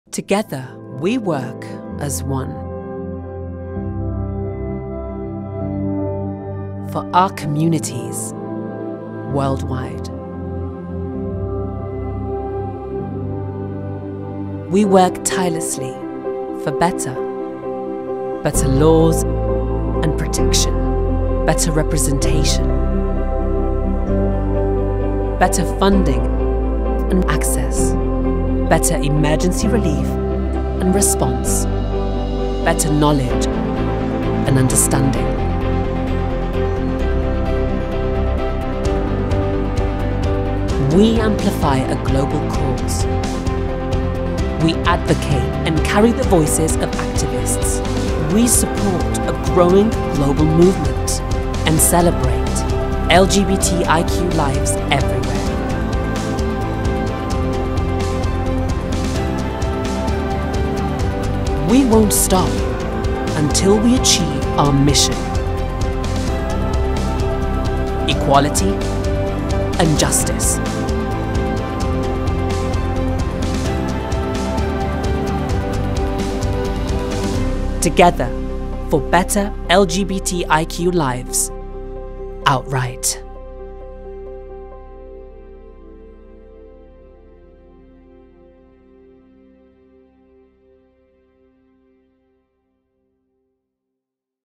Together, for Better LGBTIQ Lives. OUTRIGHT.
Outright is dedicated to working with partners around the globe to strengthen the capacity of the LGBTIQ human rights movement, document and amplify human rights violations against LGBTIQ people, and advocate for inclusion and equality.

Founded in 1990, with staff in over a dozen countries, Outright works with the United Nations, regional human rights monitoring bodies and civil society partners. Outright holds consultative status at the United Nations where it serves as the secretariat of the UN LGBTI Core Group.